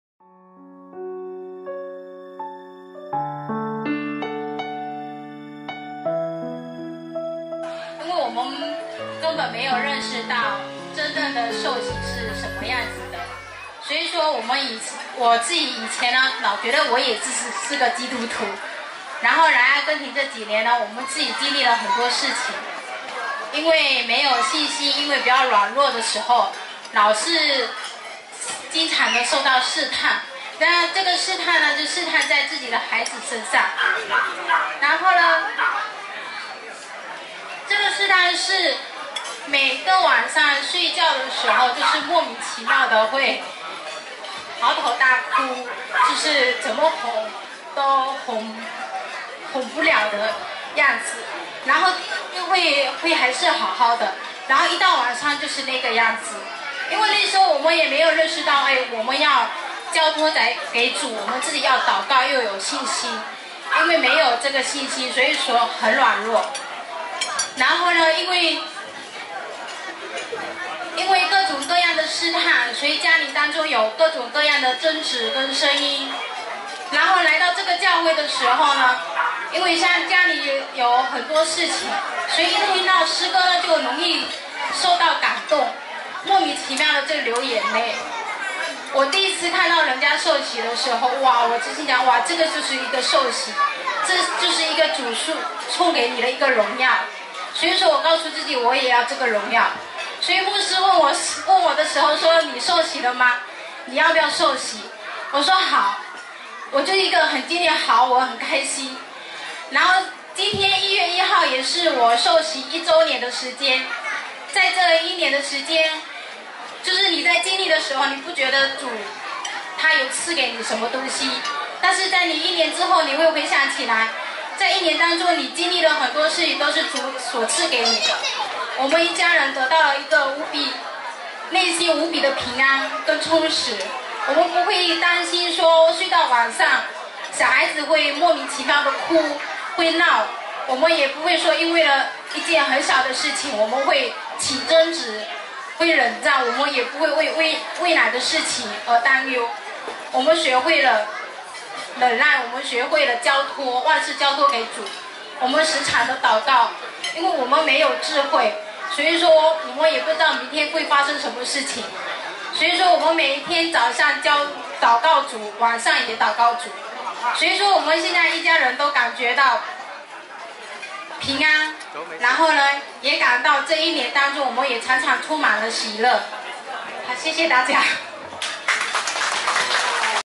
2018年元旦農莊一日游Estancia La Mimosa游小玲分享見證 阿根廷華人基督教會
2018年元旦農莊一日游Estancia La Mimosa游小玲分享見證

阿根廷華人基督教會

阿根廷 刘伟 制作